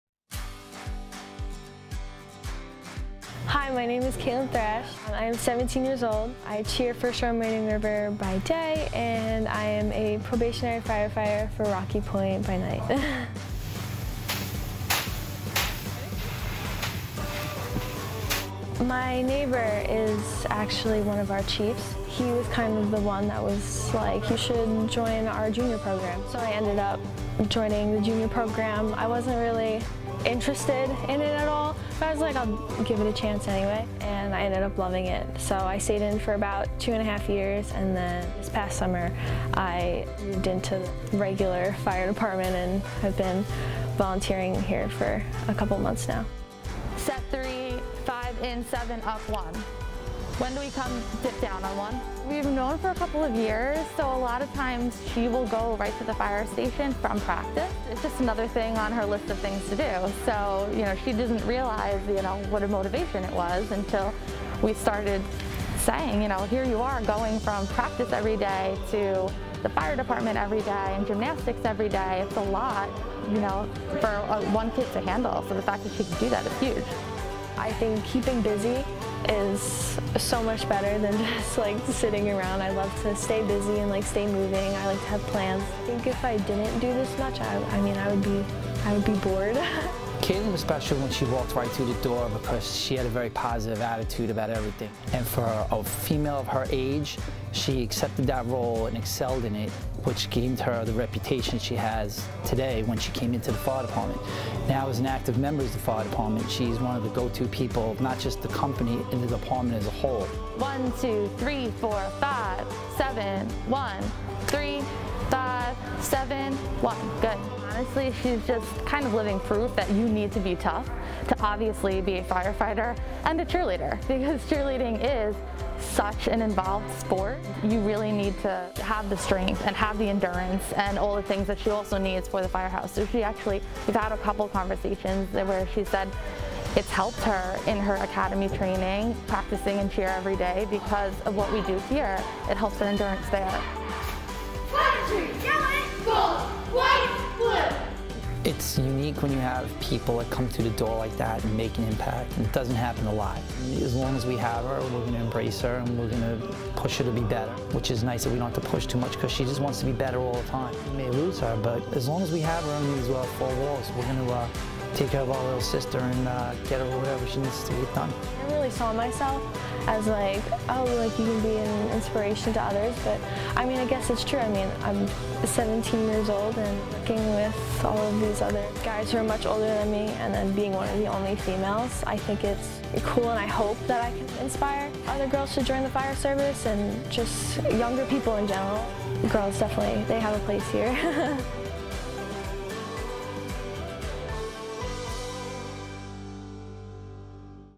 Teen cheers on a squad by day and fights fires by night as a probationary firefighter
17-year-old Caitlin Thrash lives in a neighborhood full of local Rocky Point firefighters.

Little did she know that one of her neighbors would be her current fire chief today, who encouraged her three years ago to join the Rocky Point Fire Department’s junior program.

Thrash decided to try it, and she ended up falling in love with the firehouse.

Through her involvement and commitment to the station, she leveled up to a probationary volunteer firefighter at the Rocky Point Fire Department Shoreham Company 3.

“Catlin was very special right when she walked through the door because she had a very positive attitude about everything,” said Rocky Point Chief of Department Fred Hess. “For a female of her age, she accepted that role and excelled in it, which gained her the reputation that she has today when she came inside the fire department.”

When she is not at the firehouse, you can find her practicing with her squad or cheering on the field with the Shoreham-Wading River High School Varsity cheerleading team.

“I think keeping busy is so much better than just like sitting around,” said Thrash. “I love to stay busy and stay moving. I love to have plans, and I think if I didn’t do this much, I mean I would be bored.”

Thrash’s cheerleading coach, Brie Carlen, has known about her involvement with the fire department but is still amazed at how Thrash can keep up with it all.

“Honestly, she’s just living proof that you need to be tough to obviously be a firefighter and a cheerleader,” said Carlen. “Cheerleading is such an involved sport, you really need to have the strength, the endurance, and all the things she also needs for the firehouse.”Check us out on: